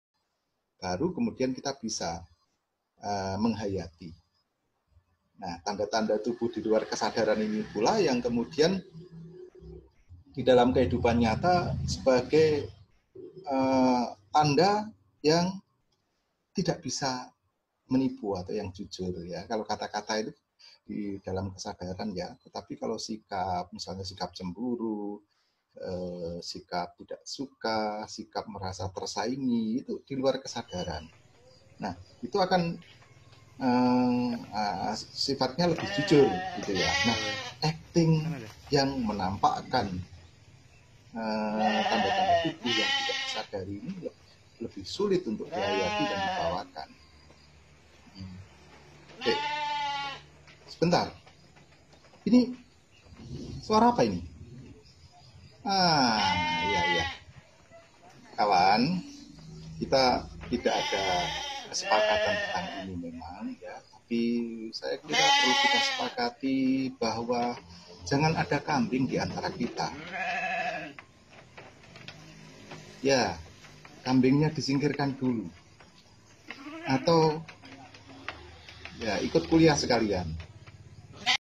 Ada kambing di kuliah daring
Resiko kuliah daring dari rumah masing2. Ada-ada saja gangguan suara.